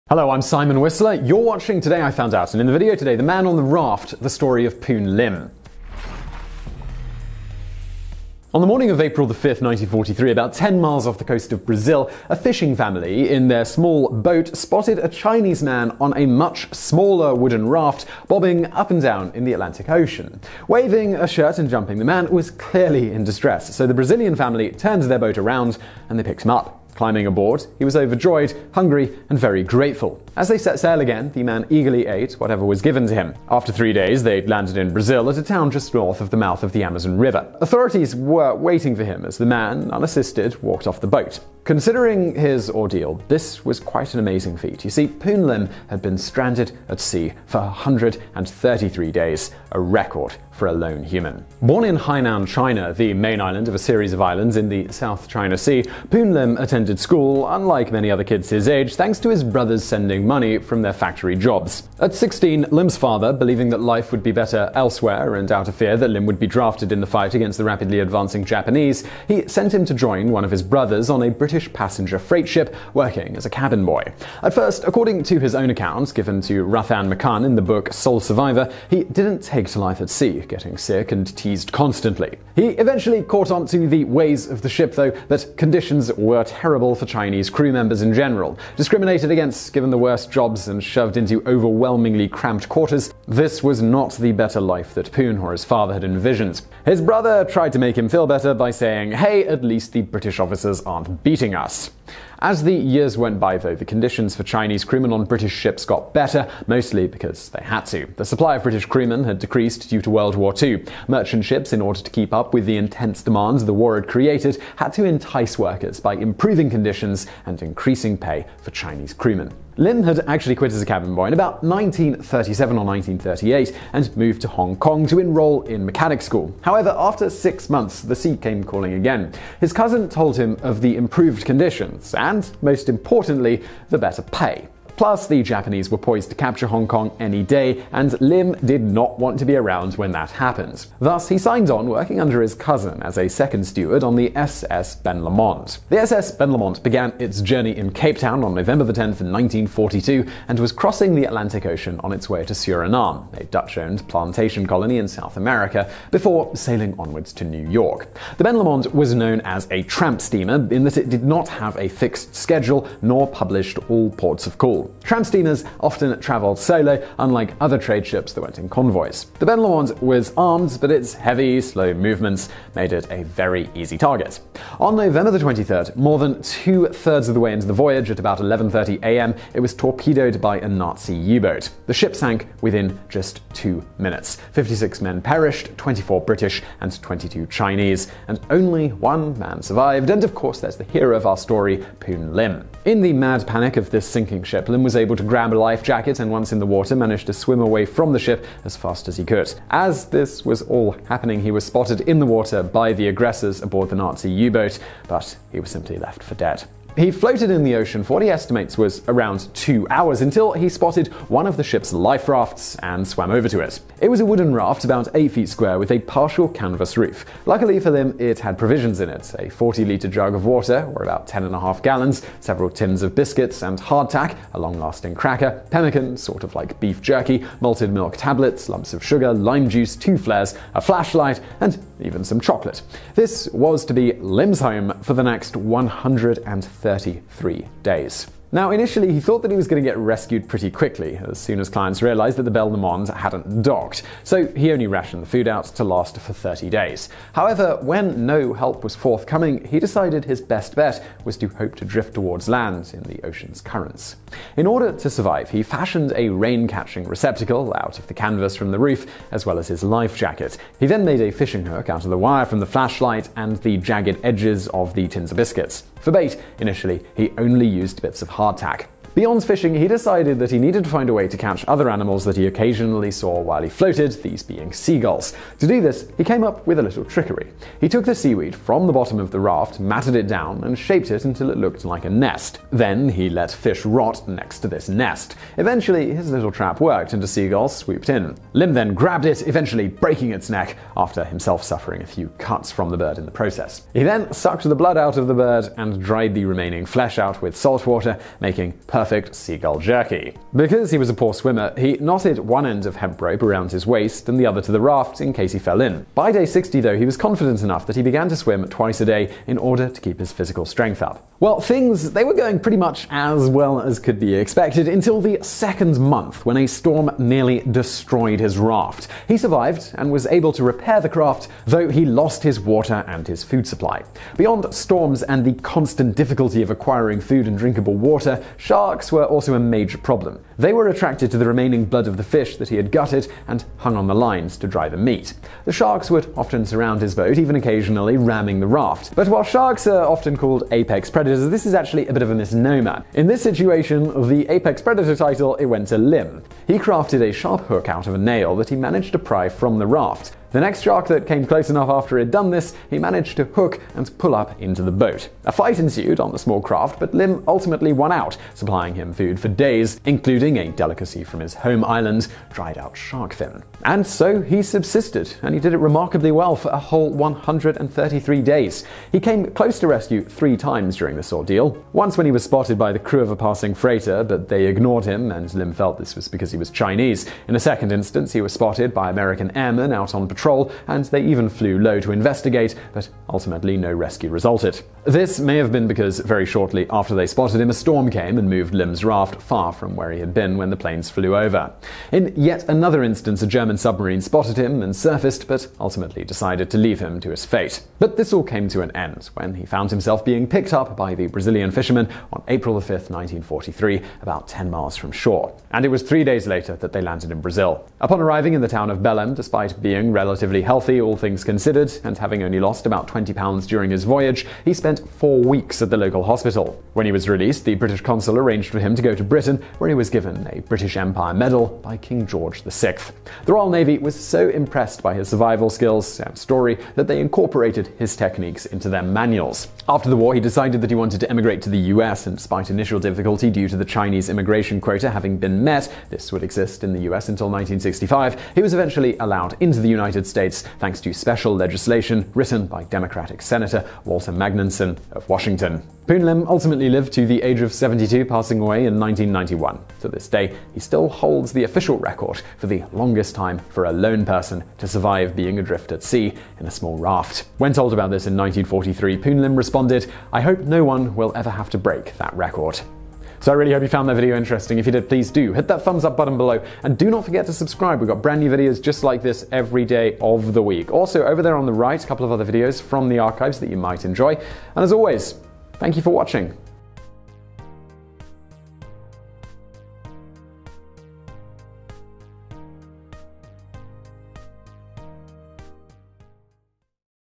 The Man on the Raft: The Story of Poon Lim
In this video:

On the morning of April 5, 1943, about ten miles off the coast of Brazil, a fishing family in their small boat spotted a Chinese man on a much smaller wooden raft bobbing up and down in the Atlantic Ocean. Waving a shirt and jumping, the man was clearly in distress, so the Brazilian family turned their boat around and picked him up. Climbing aboard, he was overjoyed, hungry, and very grateful.